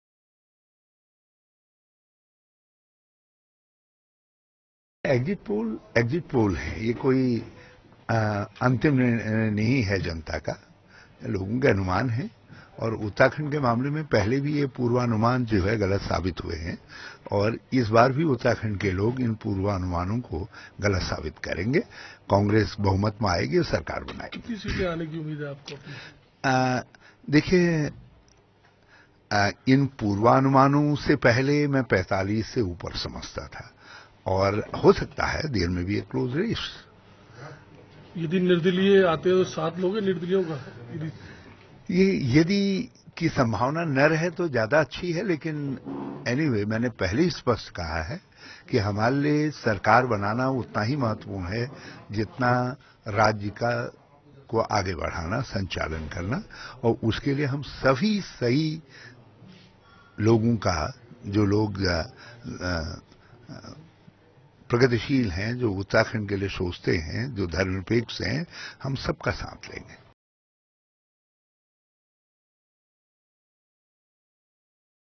"We will win again and with full majority": Harish Rawat - ANI News
Dehradun (Uttarakhand), Mar 10 (ANI): Uttarakhand Chief Minister Harish Rawat on Friday expressed confidence that the Congress party will form government will full majority. Expressing his views on the exit polls, he said, exit polls are only predictions and earlier also these predictions have proved wrong in the state.